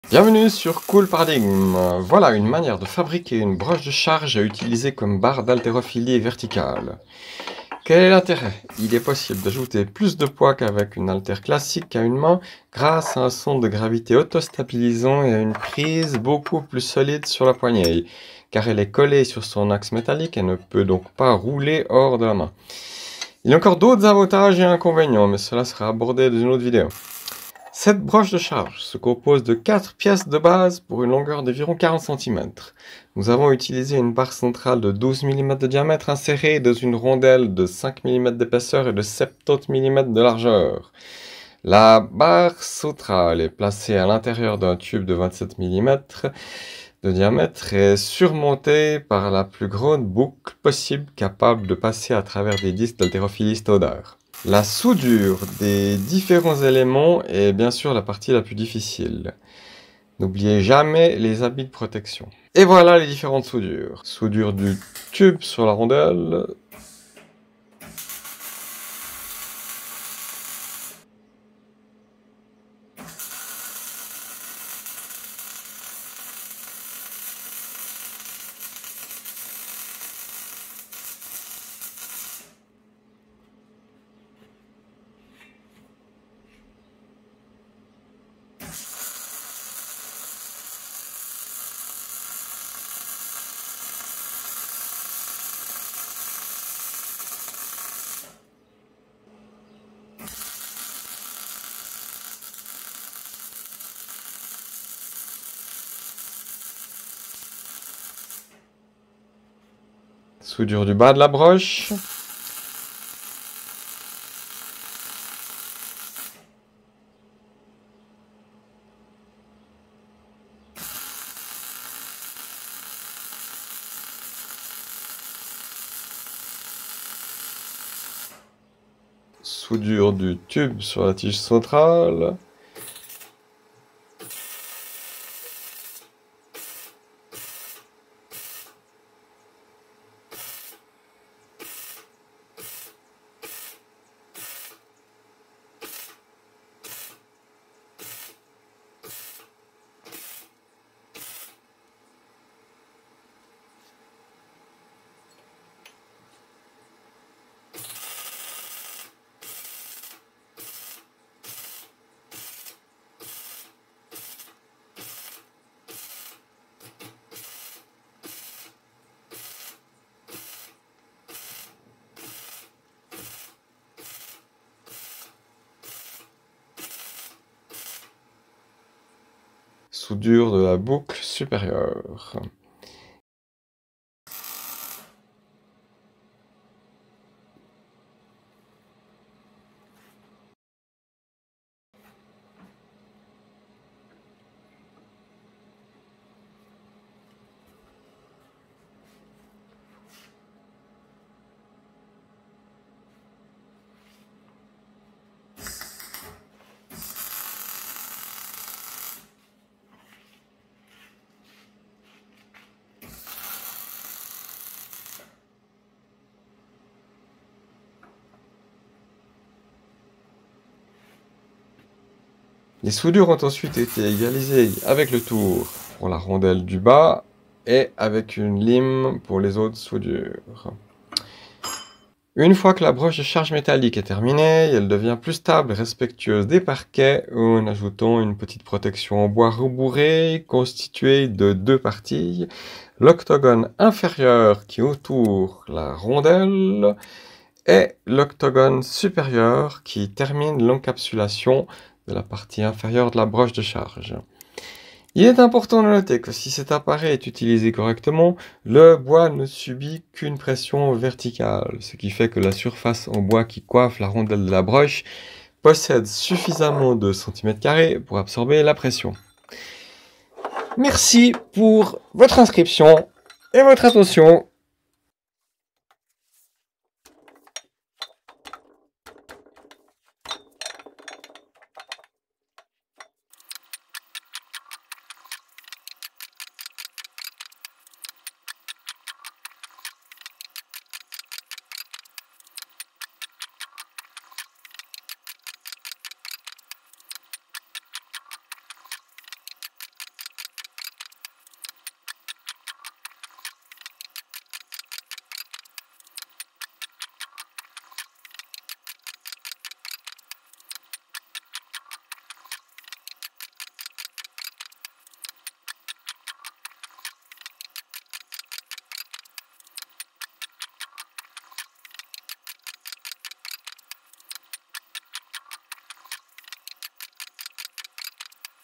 💥 🚀 Fabriquer une Haltère Verticale & 💥 étincelles surprenantes!
Il s'agit d'une 🏋  haltère verticale à une main officiellement appelée broche de charge, un substantif qui fait plus penser à un engin de stockage ou de machine de musculation qu'à une haltère en prise libre. L'haltère verticale a plusieurs avantages sur les haltères à une main classique, à savoir : plus de charges possibles, meilleurs vecteurs de forces et meilleure prise en main sans que la poignée puisse s'échapper en roulant! Et ne parlons même pas des records de power lifting à une main qui se font avec des barres prévues pour deux mains avec pour conséquence un énorme risque de déséquilibre de la barre! Évidemment mettre presque 300kg sur une broche de charge implique que la pile de disques est trop haute, ce qui a pour conséquence qu'il faudrait utiliser un podium de lifting approprié pour pouvoir mettre la poignée à une hauteur conventionnelle, ceci dit les conditions plus physiologique de l'exercice permettrait très certainement aux athlètes de battre leur propres records, à bon entendeurs 😎!

# Le tube est zingué, alors malgré le passage à la meule de la surface extérieure à souder il reste suffisamment de zinc à l'intérieur du tube pour engendrer une soudure légèrement explosive et gâcher l’esthétique des cordons, d'où la gaine thermorétractable posée par la suite pour masquer la soudure.
# Précision : la rondelle du bas de la broche fait 5.8mm d'épaisseur.